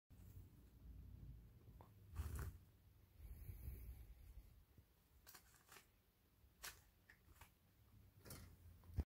POV Guinea Pig Gets Jealous
Vanna is never okay with sharing the lovies, but other piggies need love, too!